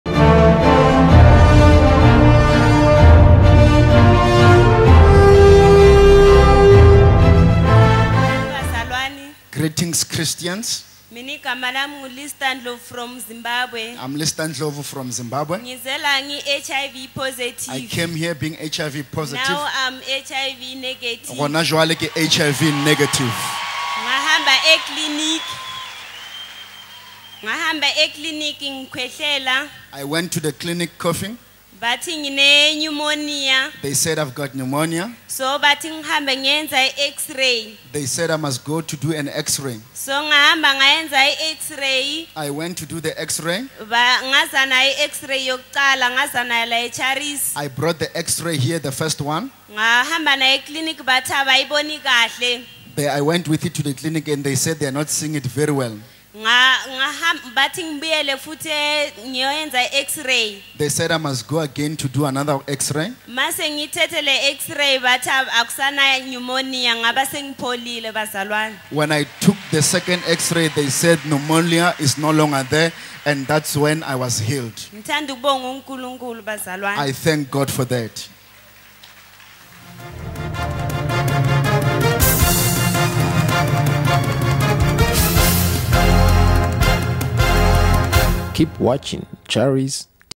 HIV HEALING ll TESTIMONY
After receiving prayers at Charis Missionary Church, this lady came back to testify the Goodness of God as she was healed from HIV and Pneumonia. Jesus is still willing to heal every sickness and cure every disease. Praise God.